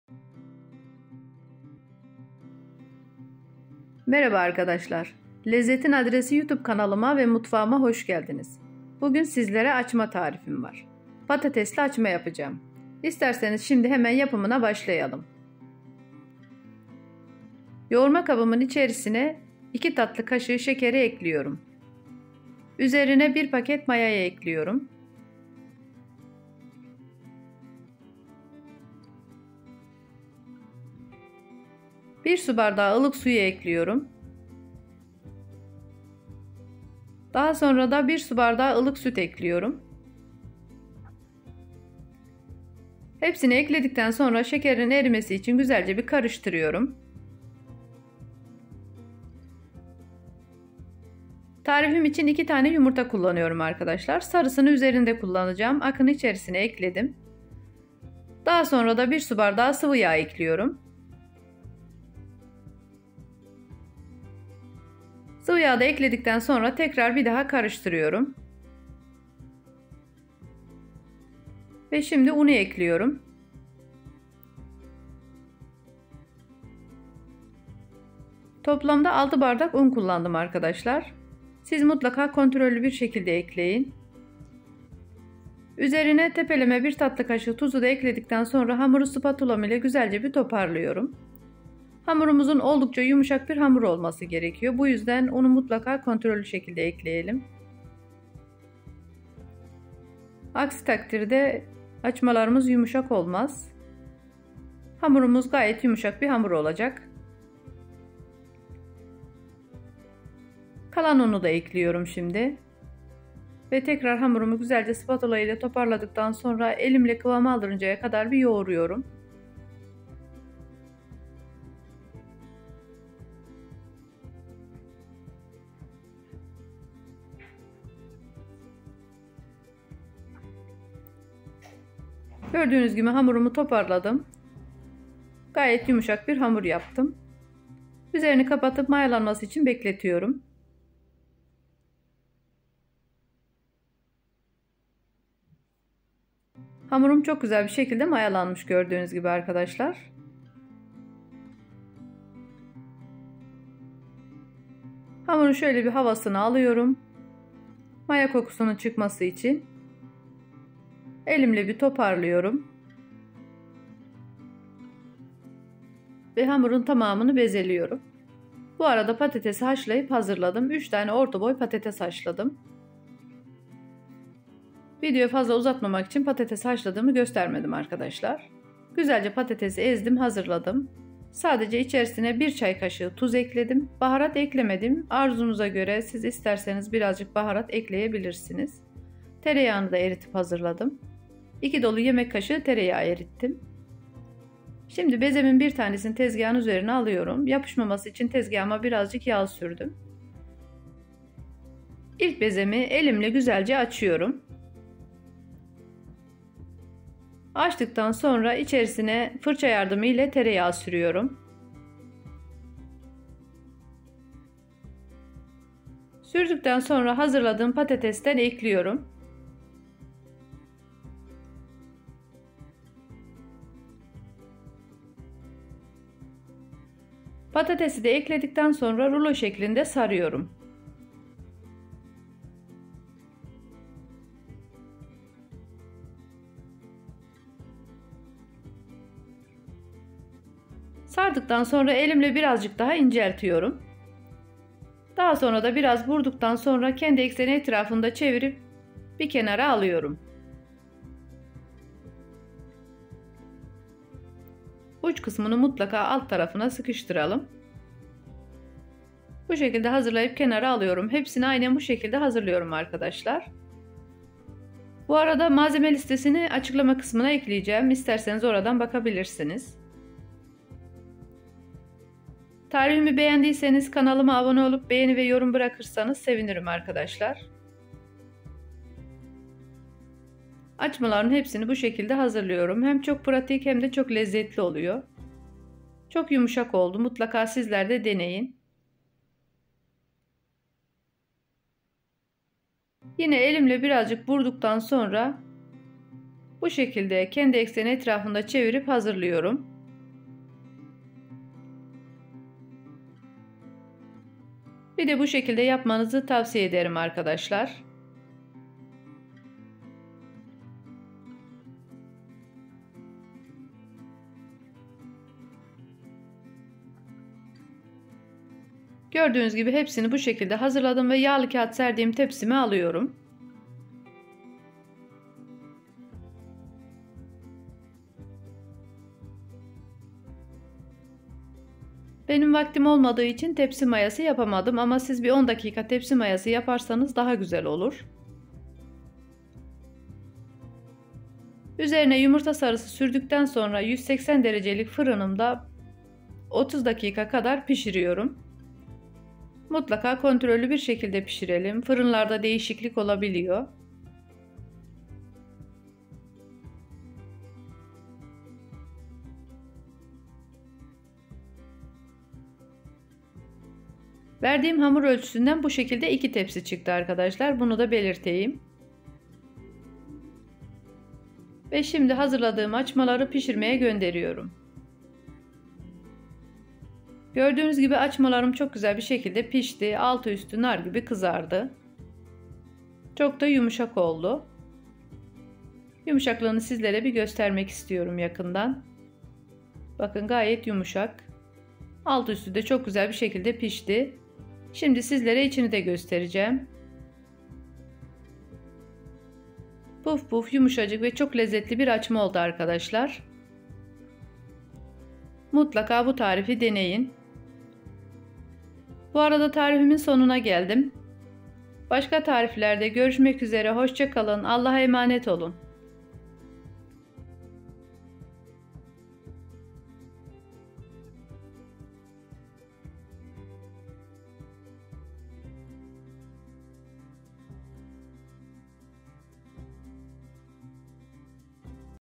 🔴BİLDİĞİNİZ AÇMALARA BENZEMEZ❗ ARTIK 1 ÖLÇÜ YETMİYOR😳TEL TEL AYRILAN YUMUŞACIK POĞAÇA AÇMA TARİFİ
Herkese merhaba çok değerli izleyenlerim. Bugün sizlere yumuşacık tel tel ayrılan patatesli poğaça tarifi hazırladım. Ben bu tarifi yapmaktan hiç sıkılmıyorum😊 ve artık 1 ölçü yapmak da yetmiyor. O kadar güzel ve lezzetli ki bu poğaçayı mutlaka denemelisiniz, yumuşaklığına hayran kalacaksınız.

MALZEMELER
250 ml ılık süt
200 ml ılık su
200 ml sıvı yağ
2 tatlı kaşığı şeker
1 tatlı kaşığı tuz
1 paket maya (10 gr)
2 tane yumurta (sarısı üzerine akı içine kullanılacak)
6 bardak un

iç harcı için
3 tane orta boy patates
bir çay kaşığı tuz

arasına sürmek için
2 yemek kaşığı tereyağı

Patatese baharat eklemedim arzunuza göre biraz karabiber ve birazcık da pul biber ekleyebilirsiniz.

Tarifimi beğendiyseniz düşüncelerinizi benimle paylaşmayı ve beğeni bırakmayı unutmayınız❤

Materials
250 ml of warm milk
200 ml of warm water
200 ml of oil
2 teaspoons of sugar
1 teaspoon of salt
1 packet of yeast (10 g)
2 eggs (to be used in the whites on the yolks)
6 cups flour

for stuffing
3 medium-sized potatoes
teaspoon salt

to drive between Dec
2 tablespoons of butter

I did not add spices to the potato, you can add a little black pepper and a little chili pepper according to your desire.

Materialien
250 ml warme Milch
200 ml warmes Wasser
200 ml Öl
2 Teelöffel Zucker
1 Teelöffel Salz
1 Packung Hefe (10 g)
2 Eier (mit Eigelb)
6 Tassen Mehl

für Inneren Mörtel
3 mittelgroße Kartoffeln
ein Teelöffel Salz

zwischen ihnen zu fahren
2 Esslöffel Butter

Ich habe keine Gewürze zu den Kartoffeln hinzugefügt, Sie können etwas schwarzen Pfeffer und ein wenig Chilischoten hinzufügen, je nach Wunsch.

Wenn dir mein Rezept gefällt, vergiss nicht, deine Gedanken mit mir zu teilen und mir zu gefallen❤

Materiallar
250 ml isti süd
200 ml isti su
200 ml yağ
2 çay qaşığı şəkər
1 çay qaşığı duz
1 paket maya (10 q)
2 yumurta (sarıya deyil, ağlara çevriləcək)
6 stəkan un

daxili həll üçün
3 orta kartof
bir çay qaşığı duz

Dec arasında sürmək üçün
2 xörək qaşığı kərə yağı

Kartofa ədviyyat əlavə etmədim, istədiyiniz kimi bir az qara bibər və bir az Çili lopa əlavə edə bilərsiniz.

Reseptimi bəyəndinizsə, fikirlərinizi mənimlə bölüşməyi və bəyənməyi unutmayın❤